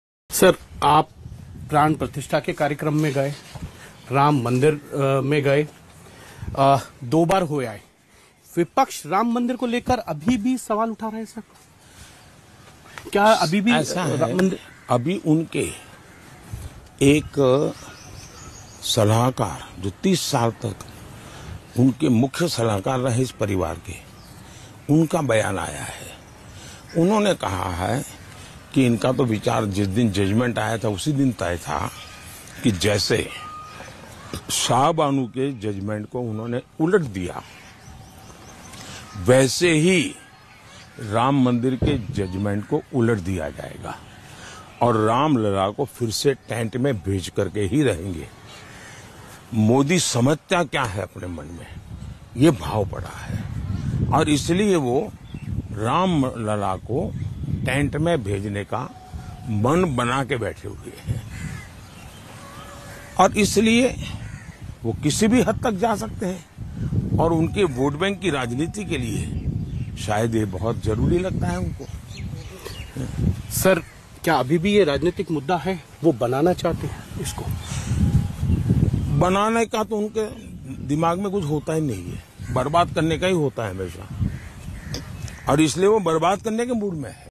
Congress’ intentions was to always see ‘Ram Lalla’ in a tent: PM Modi
In an interview to Aaj Tak, Prime Minister Narendra Modi said that Congress’ intentions was to always oppose the judgement on the Pran-Pratishtha of Shri Ram Mandir and to see Ram Lalla in tent. He added that for the politics of vote-bank, Congress boycotted the Pran-Pratishtha of Shri Ram Mandir.